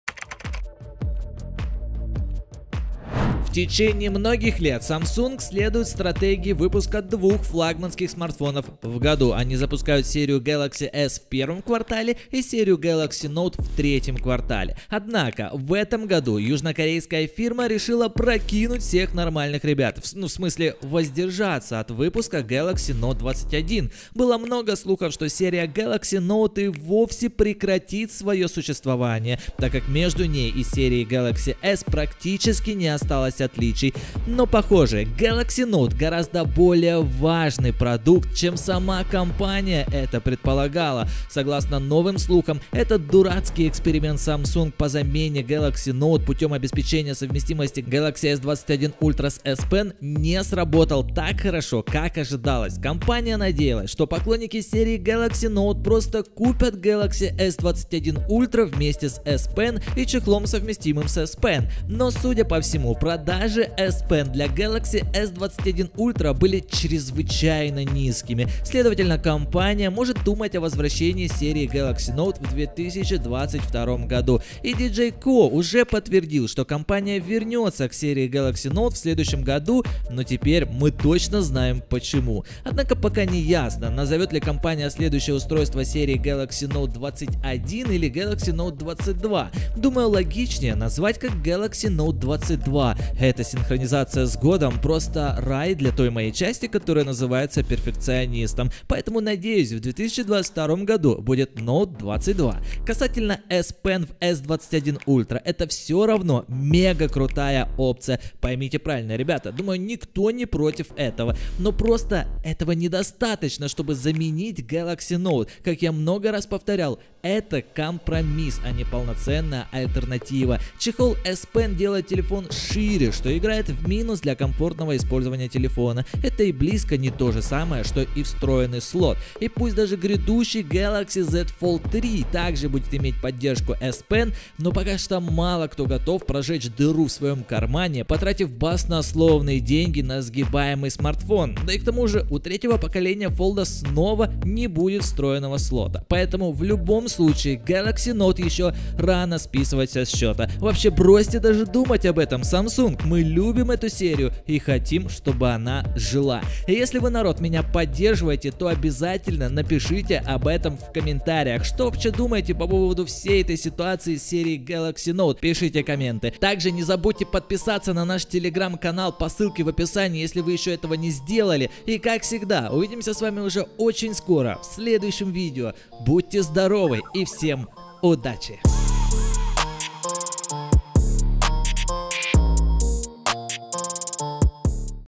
Samsung Galaxy Note 22 - ЭТО БУДЕТ ГРАНДИОЗНО!
В течение многих лет Samsung следуют стратегии выпуска двух флагманских смартфонов. Они запускают серию Galaxy S в первом квартале и серию Galaxy Note в третьем квартале. Однако в этом году южнокорейская фирма решила воздержаться от выпуска Galaxy Note 21. Но судя по всему, продажи S Pen для Galaxy S21 Ultra были чрезвычайно низкими. Следовательно, компания может подумать о возвращении серии Galaxy Note в 2022 году.

И я стану еще счастливей, если вы подпишетесь на меня в